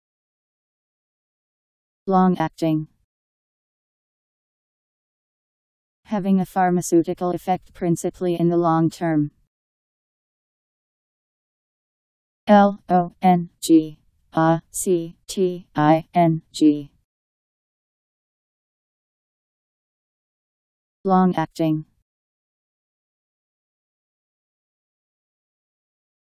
Long-acting Meaning
Video shows what long-acting means. Having a pharmaceutical effect principally in the long term..  Long-acting Meaning. How to pronounce, definition audio dictionary. How to say long-acting. Powered by MaryTTS, Wiktionary